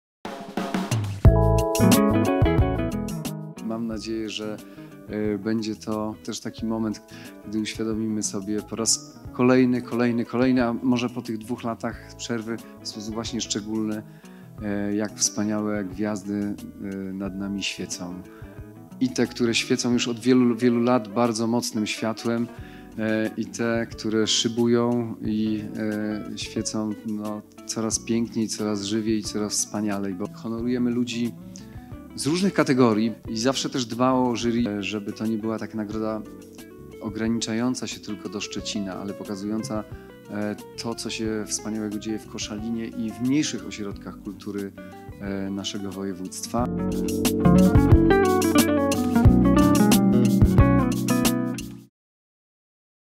Zachodniopomorscy artyści docenieni!
To był wyjątkowy wieczór! 29 października w Operze na Zamku w Szczecinie odbyła się jubileuszowa, dziesiąta gala rozdania nagród Marszałka Województwa Zachodniopomorskiego „Pro Arte”! Tym razem laureatów było aż ośmioro, ponieważ uhonorowano także osoby, którym przyznano to wyróżnienie w poprzednim roku (wówczas ceremonia nie została zorganizowana).